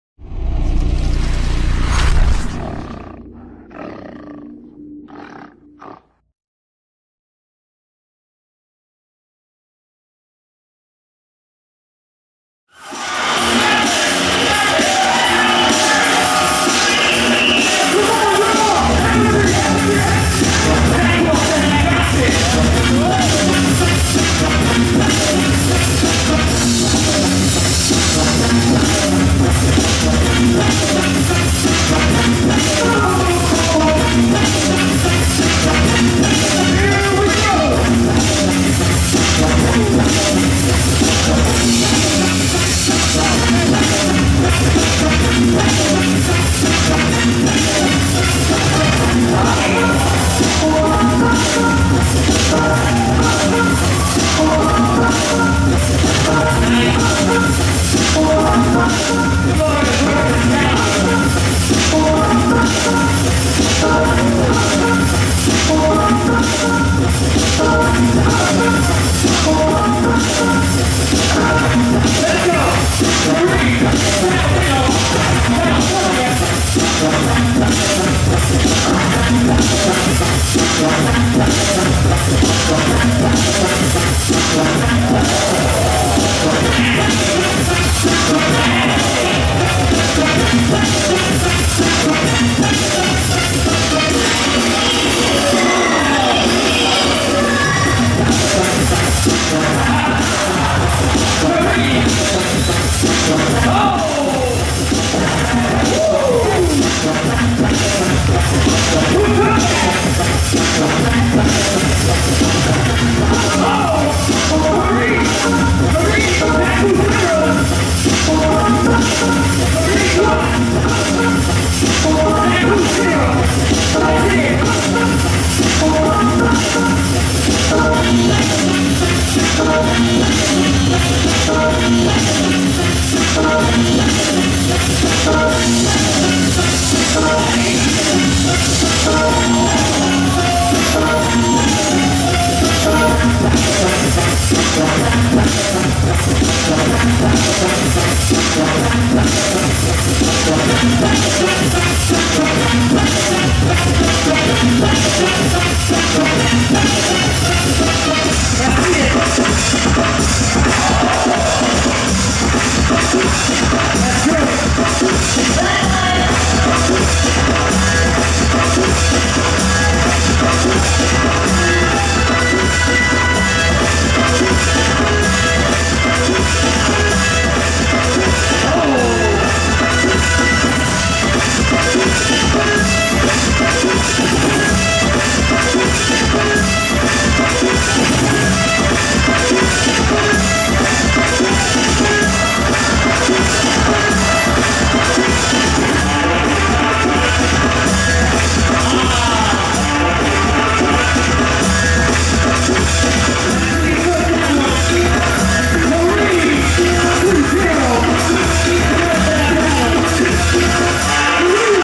Zeku Vs Kareem | 1 V 1 Top 16 | Silverback Open 2015 | Pro Breaking Tour | BNC
Silverback Open Championships 2015

1 V 1 Top 16

Winner
Kareem (Rock Force)

Judges
Ken Swift (7 Gems)
Wicket (Renegade Rockers)
Poe-1 (Style Elements)
Yan The Shrimp (All The Most)
Free (Circle Of Fire)
Ghost (Lionz Of Zion)
Focus (Flomo)

Djs
Foxx Boogie (Repstyles)
Blesone (Massive Monkees/Mashhall/TheM Team)
Fleg (Lionz Of Zion)
Scream

Emcee
Nemesis (Breaks Kru)